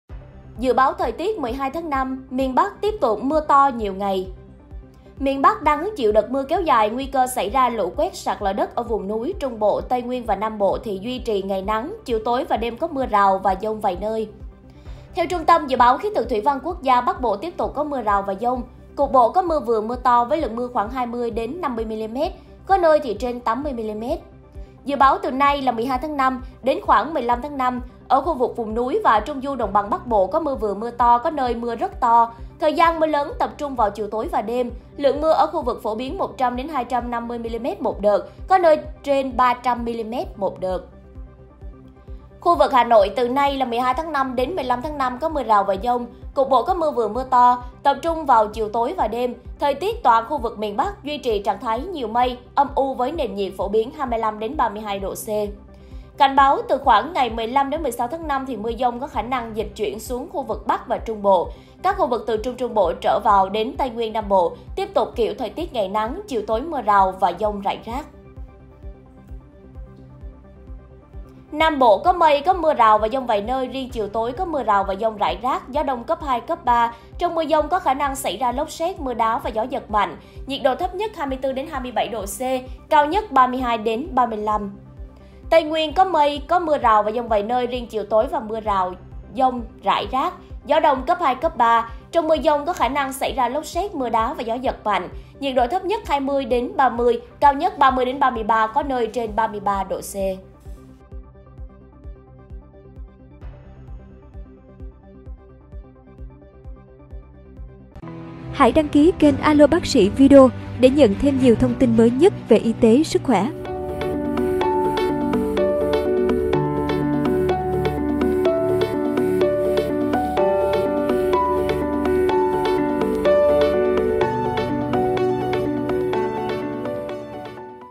Dự báo thời tiết 12/5: Miền Bắc tiếp tục mưa to nhiều ngày
Dự báo thời tiết ngày 12/5, miền Bắc đang hứng chịu đợt mưa kéo dài, nguy cơ xảy ra lũ quét, sạt lở đất ở vùng núi. Trung Bộ, Tây Nguyên và Nam Bộ duy trì ngày nắng, chiều tối và đêm có mưa rào và giông vài nơi.👉 Tin tức COVID-19 mới nhất

MỌI GÓP Ý XIN GỬI VỀ:
Hợp tác nội dung:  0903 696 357